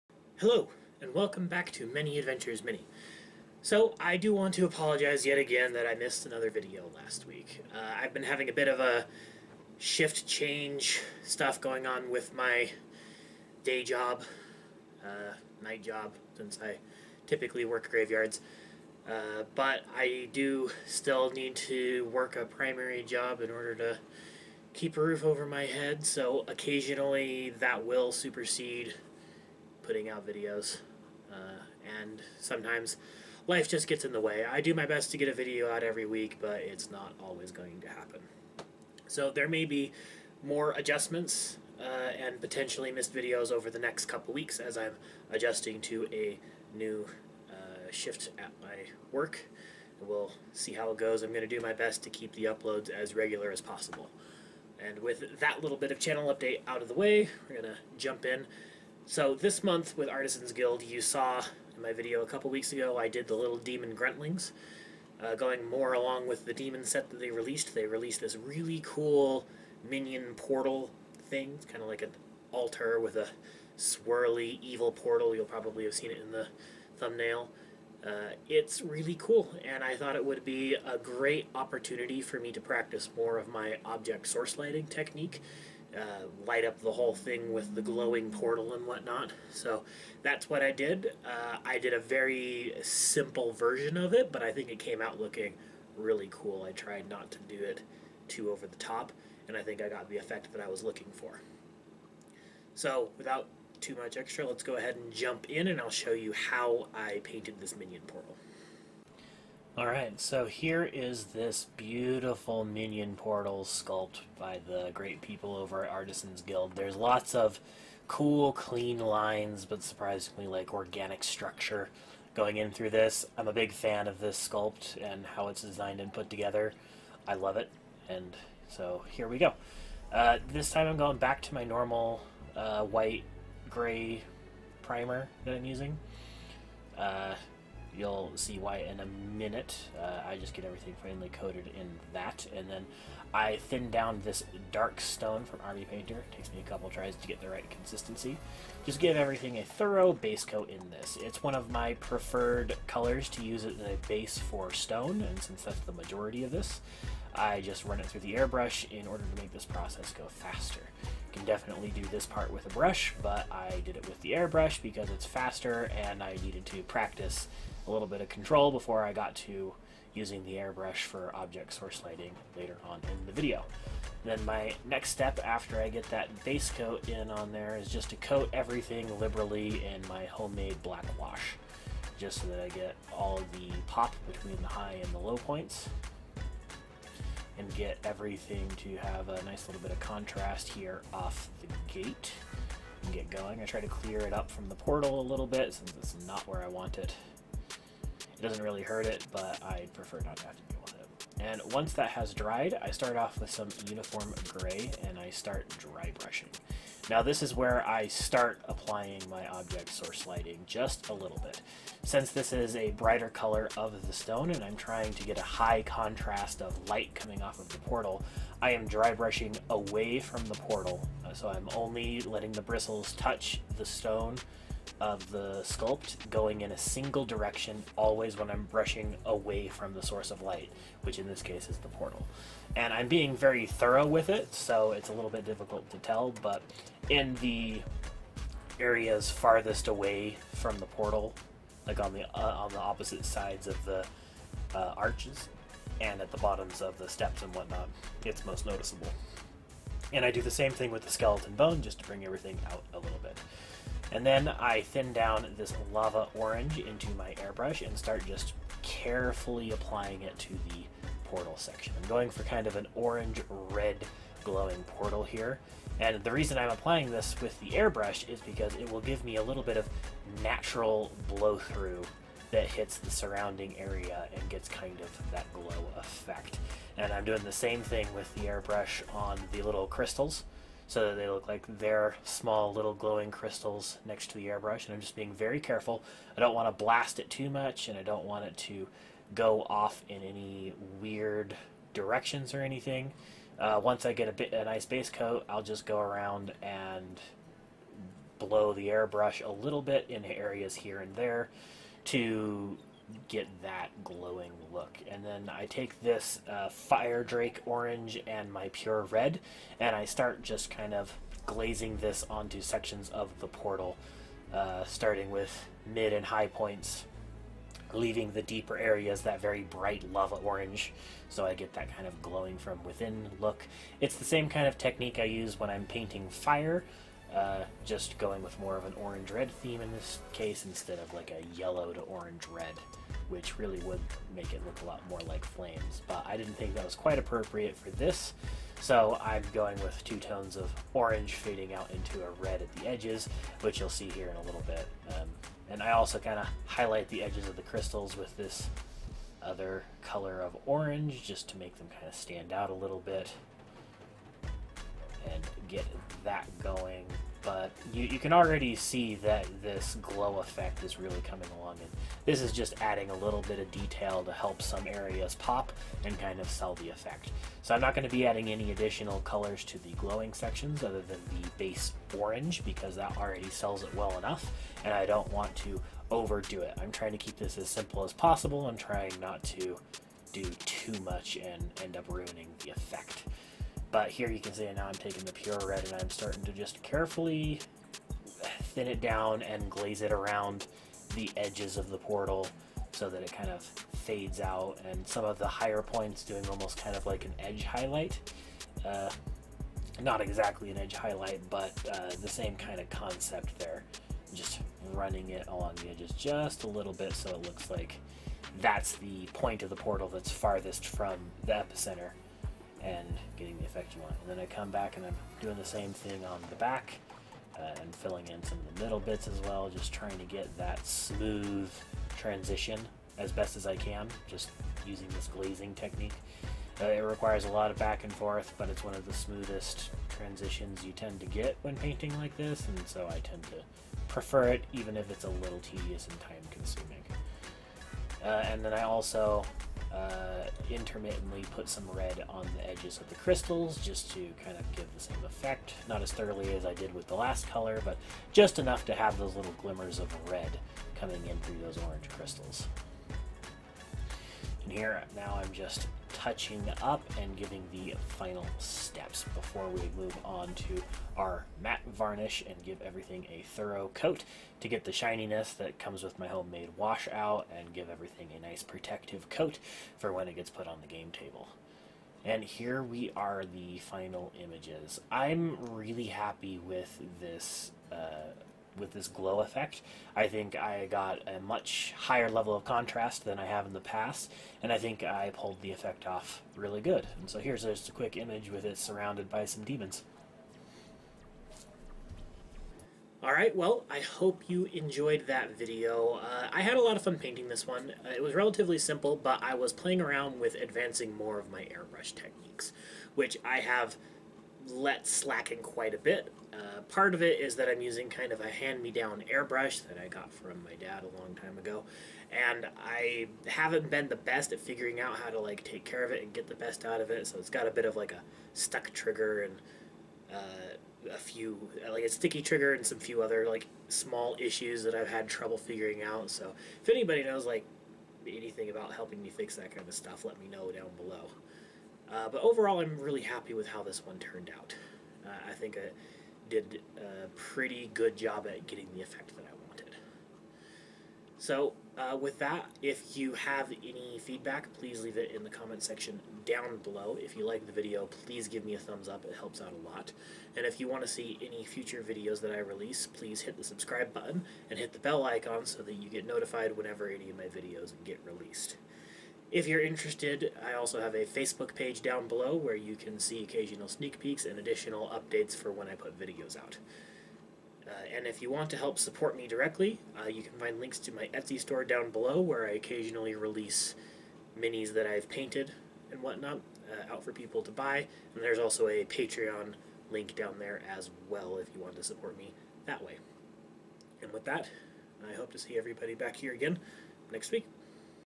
Painting the Minion Portal from Artisans Guild
I'm continuing to consort with demons this week by painting up this demonic Minon Portal to allow the creatures of the abyss access to the mortal plane.

Also check out my Etsy store if you want to buy some of the things I make to put up for sale